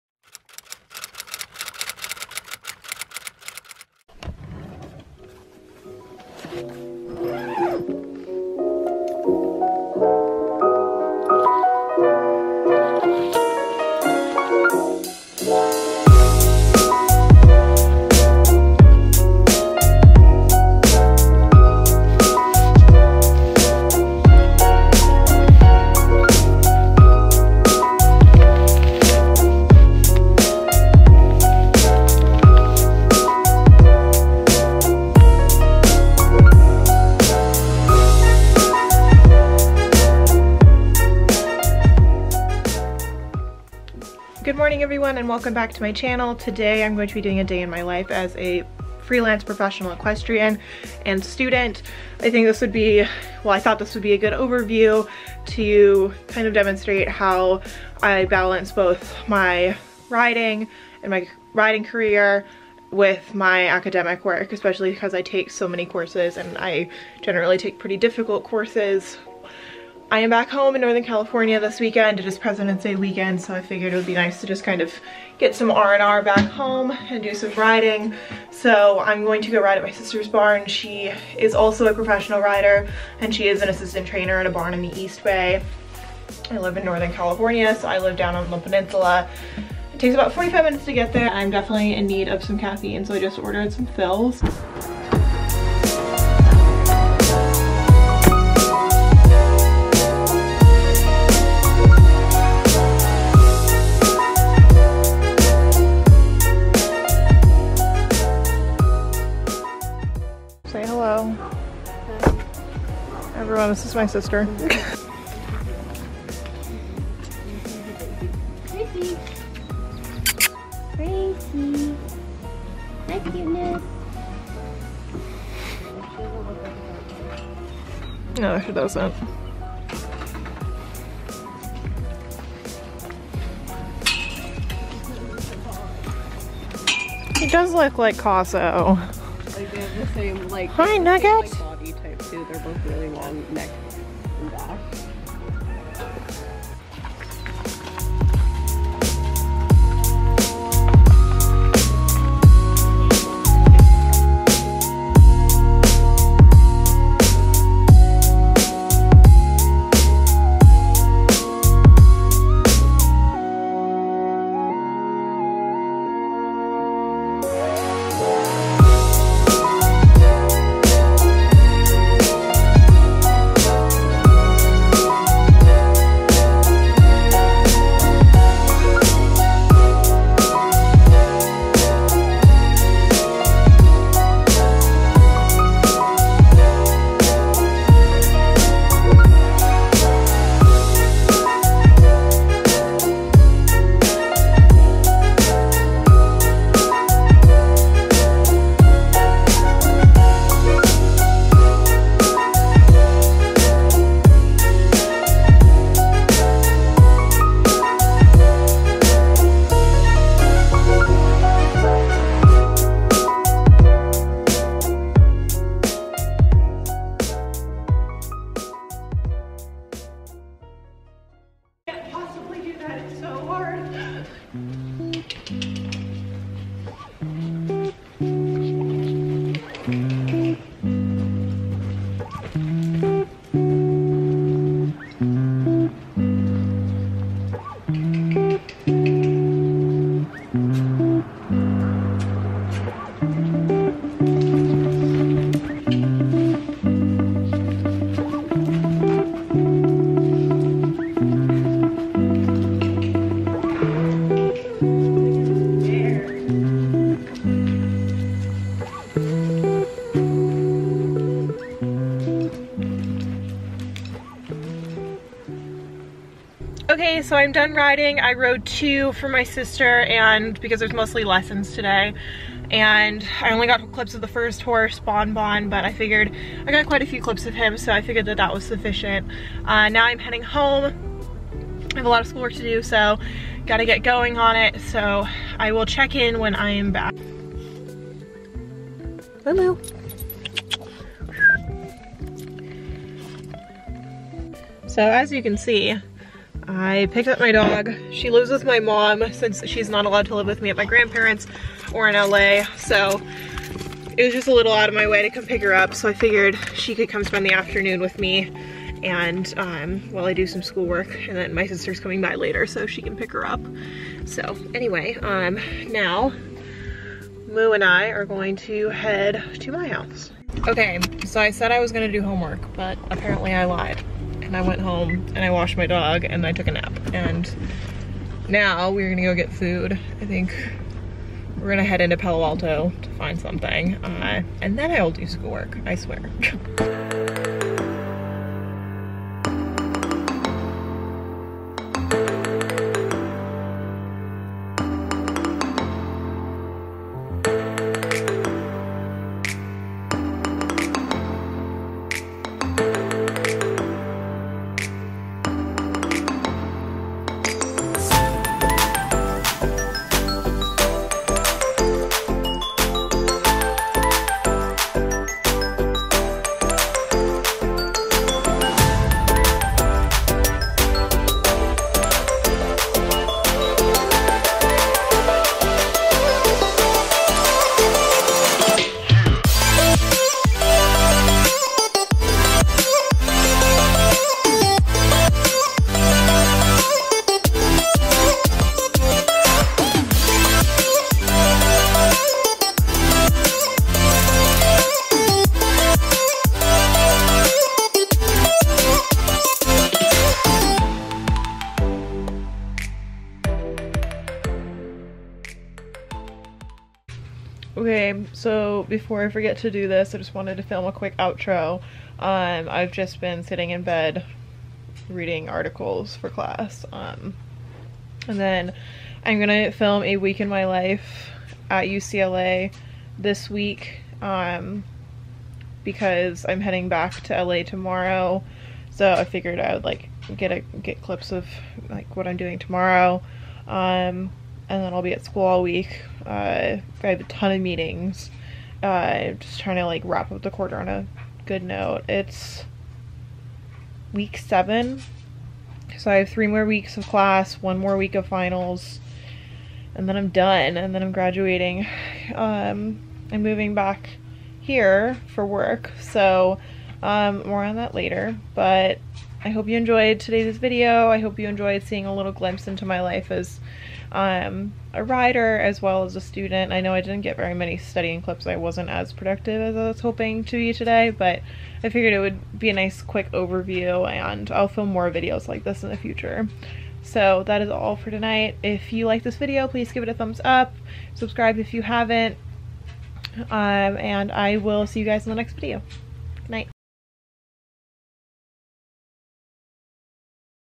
Day in My Life as a UCLA Transfer Student and Professional Equestrian | Vlog
Hey everyone! This is a short video of an easy day of riding/training, hanging out with my sister, and completing some schoolwork. Balance is key to having a positive life and I try my best to stay on top of all my responsibilities while having fun. I hope you enjoyed! UCLA